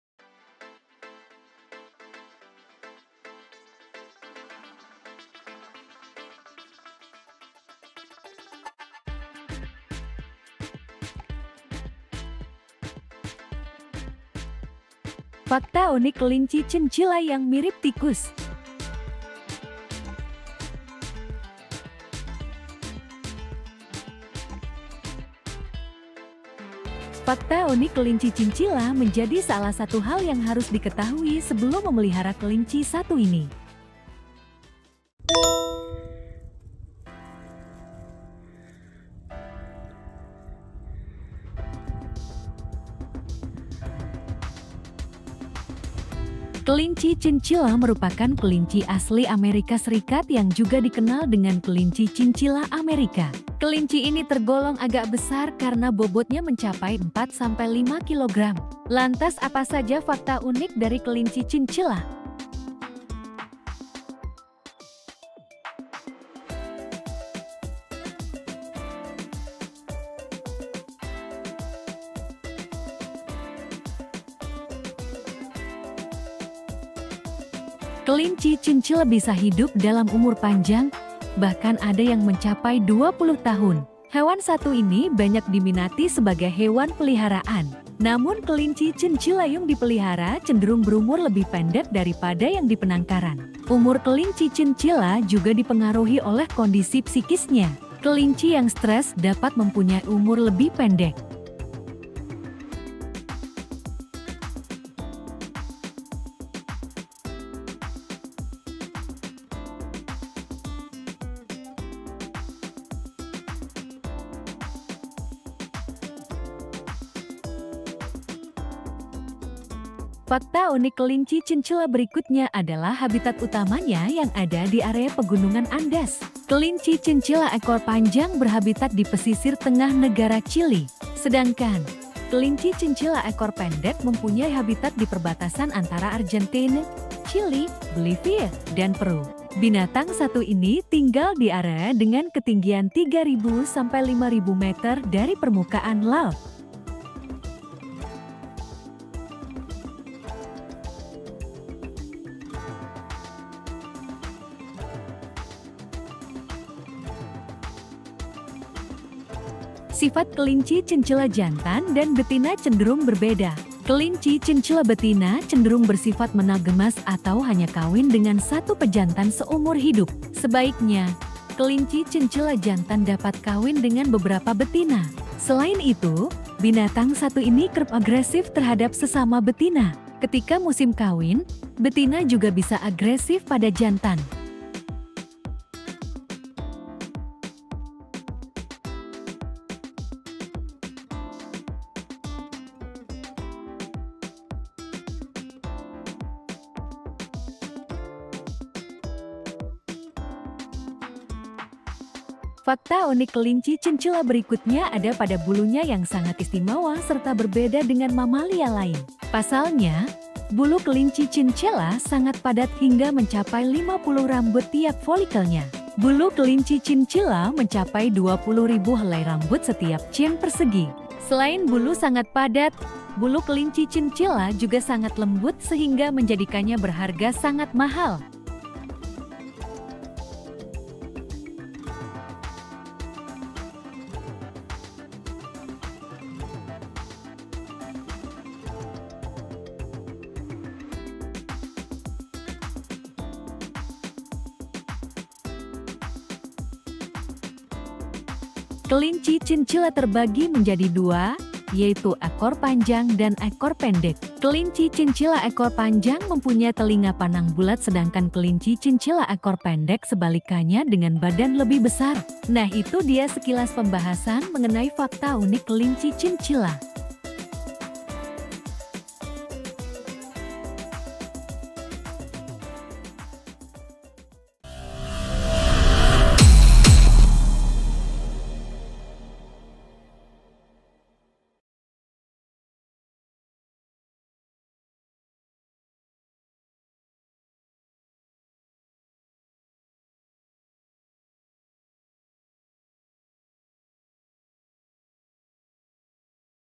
5 Fakta Unik Kelinci Chinchilla yang Mirip Tikus.
5 Fakta Unik Kelinci Chinchilla yang Mirip Tikus.

kelinci chinchilla bisa hidup dalam umur panjang, bahkan ada yang mencapai 20 tahun. Hewan satu ini banyak diminati sebagai hewan peliharaan. Namun kelinci chinchilla yng dipelihara cenderung berumur lebih pendek daripada yang dipenangkaran.
Umur kelinci chinchilla juga dipengaruhi oleh kondisi psikisnya. Kelinci yang stres dapat mempunyai umur lebih pendek.

Terimakasih Sudah Bersedia Menonton...!